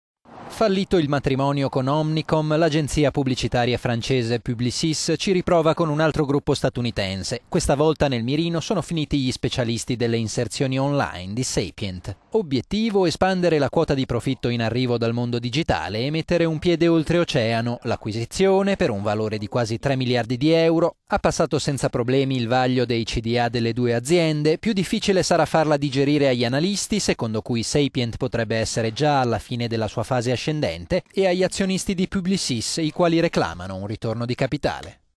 Pubblicità, Publicis si compra la statunitense Sapient - economy
Fallito il matrimonio con Omnicom, l'agenzia pubblicitaria francese Publicis ci riprova con un altro gruppo statunitense. Questa volta nel mirino sono finiti gli specialisti delle inserzioni online di Sapient.

Obiettivo: espandere la quota di profitto in arrivo dal mondo digitale e mettere un piede oltreoceano. L'acquisizione, per un valore di quasi 3 miliardi di euro, ha passato senza problemi il vaglio dei cda delle due aziende.